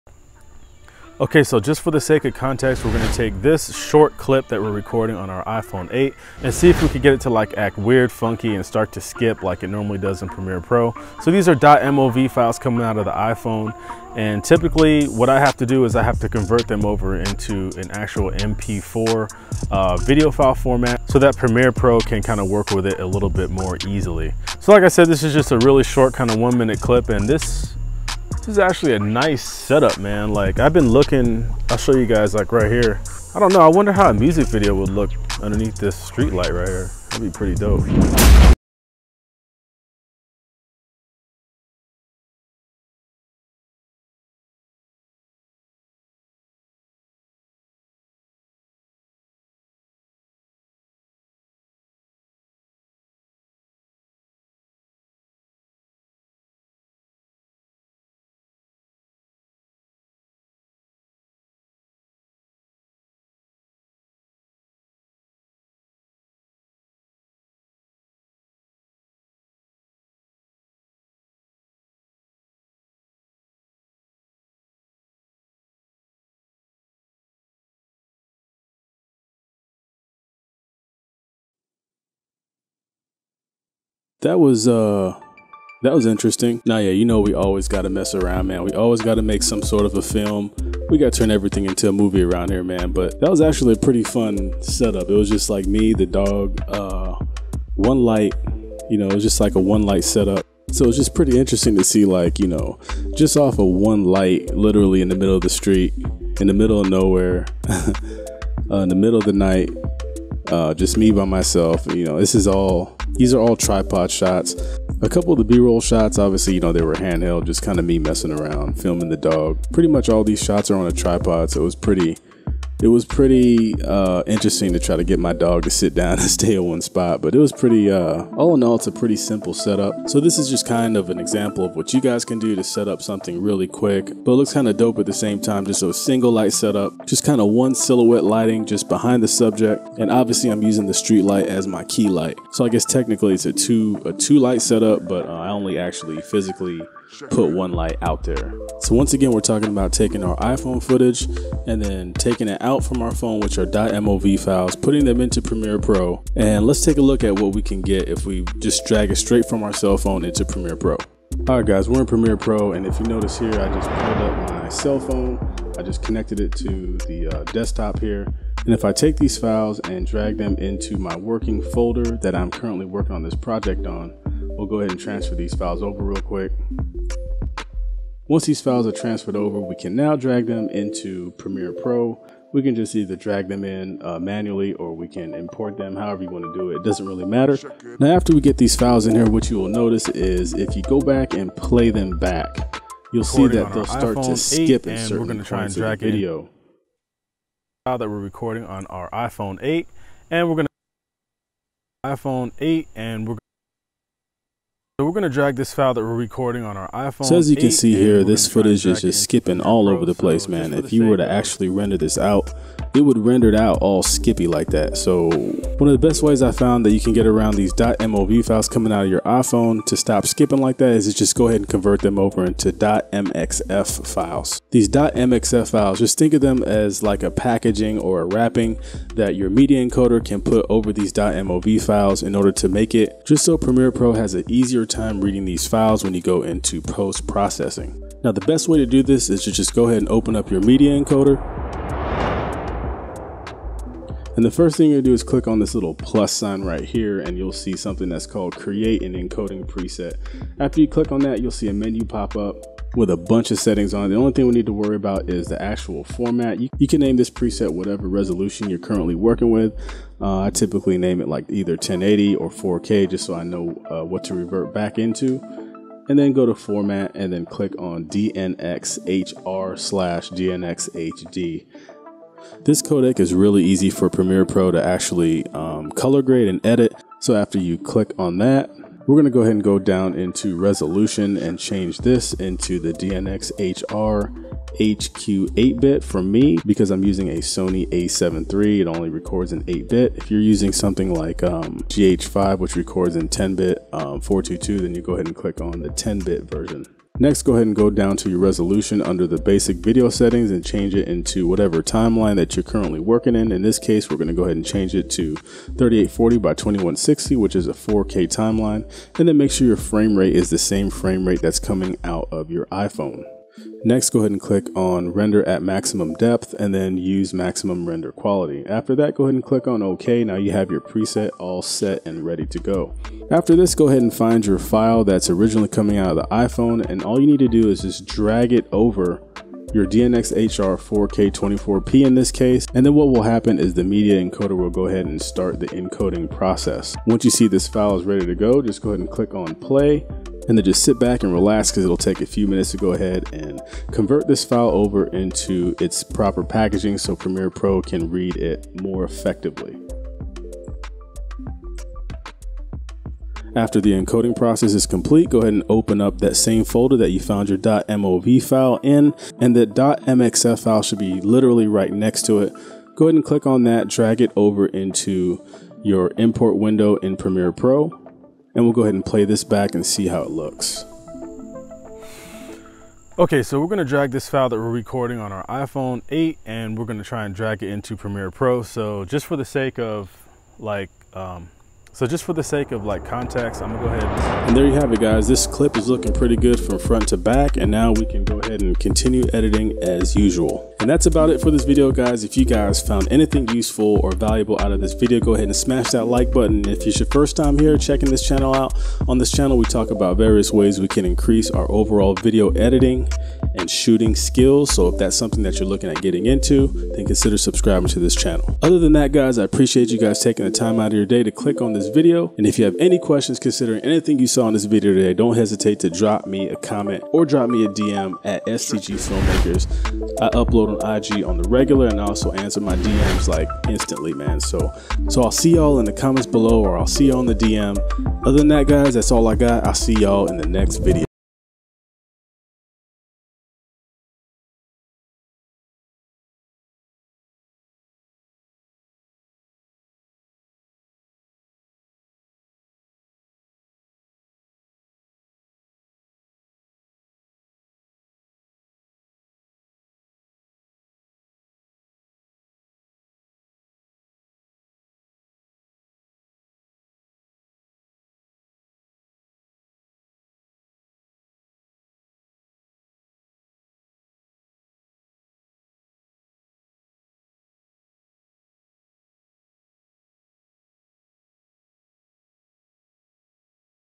iPhone Video Glitches In Premiere Pro | Import iPhone Video Premiere Pro
iPhone Video Glitches In Premiere Pro | Import iPhone Video Premiere Pro | Glitching | Skipping | Edit iPhone footage convert iPhone footage Adobe Media Encoder Adobe Premiere Pro to fix stuttering skipping.

Graded using Adobe Premiere Pro CC 2019
Website Designer: Shane Gillins @stgfilmmakers
How to support STG's future video content creating endeavors:
0:00 Intro
2:30 Tutorial
5:01 Resolution Settings
6:11 Final Render
6:31 Outro